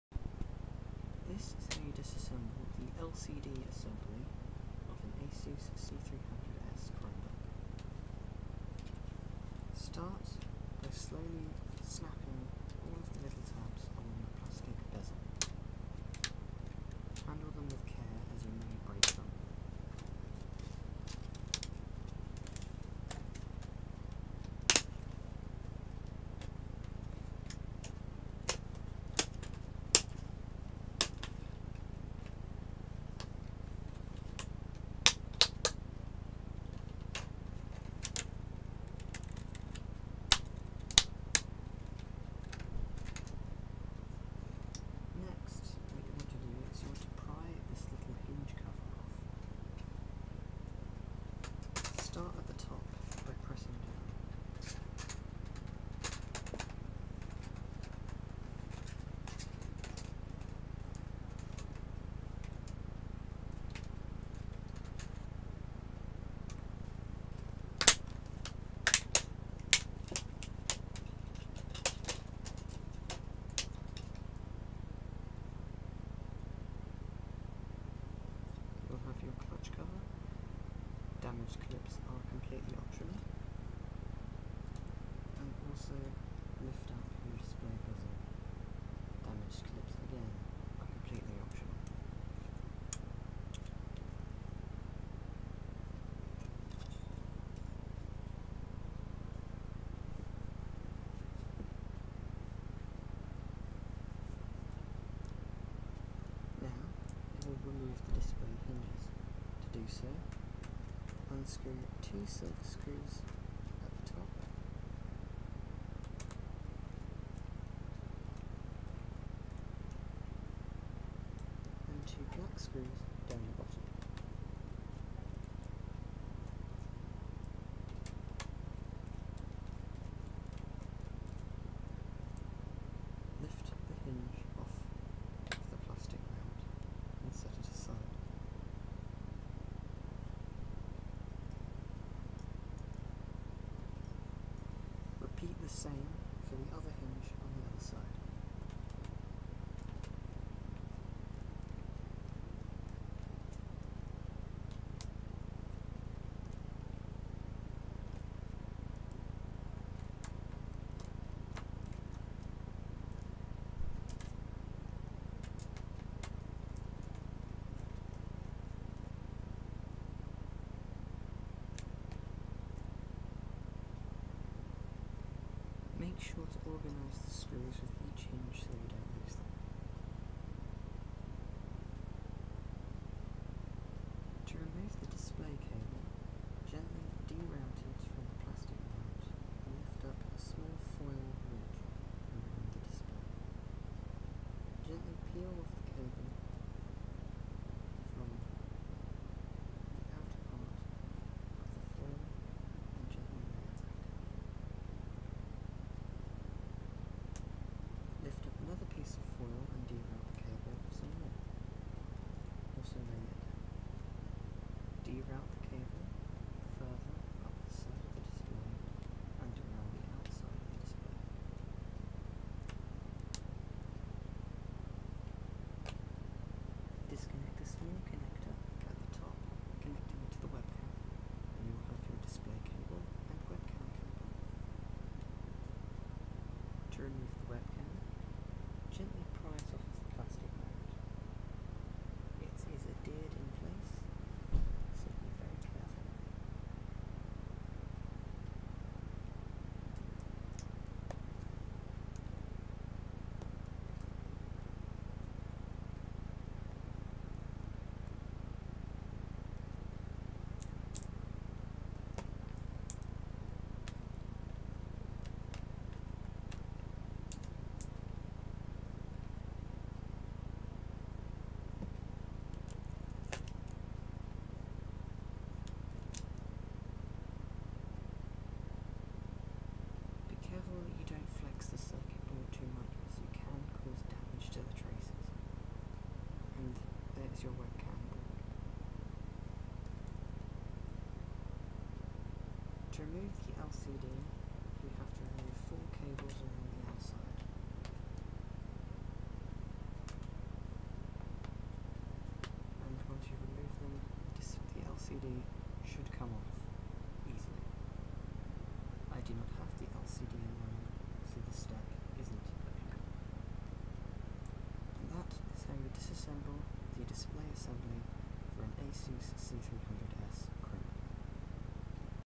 How to teardown a Asus C300 display assembly
Again scavenging parts I can sell from this thing